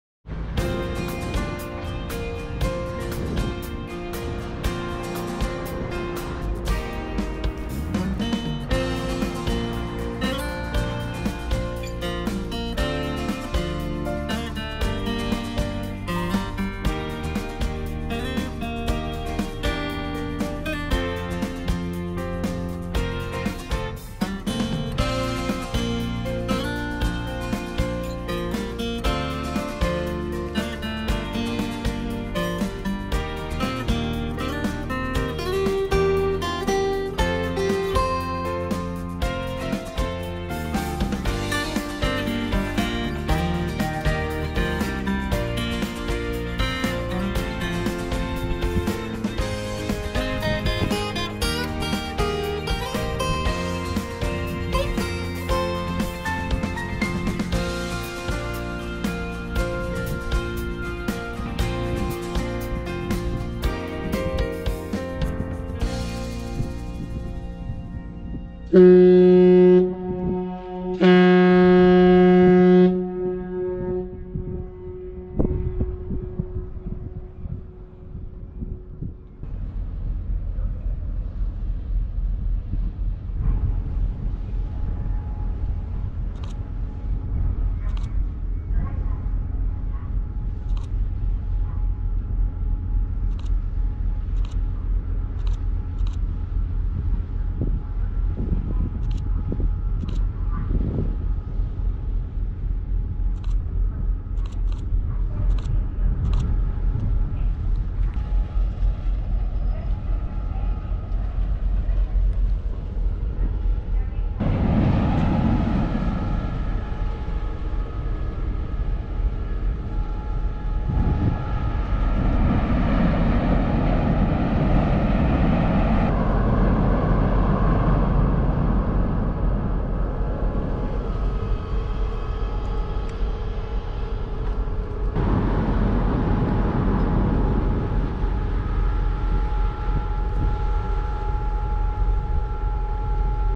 利尻島沓形フェリーターミナルでフェリー入港を見た夏の日
礼文島香深から利尻島沓形までフェリーで40分。沓形フェリーターミナルで利尻島からのフェリー入港シーンを見ました。